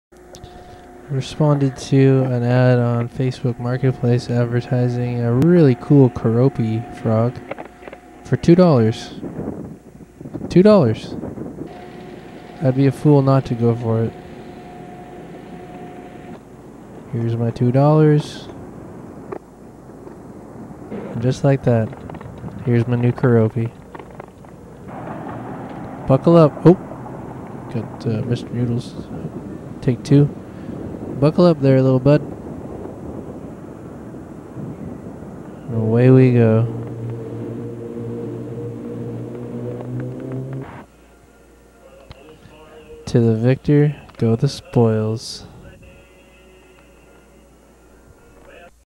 2$ keroppi
last night i went the distance for this 2$ keroppi all in all it was about 1.5 hour bike ride but i think it was worth it for this cutie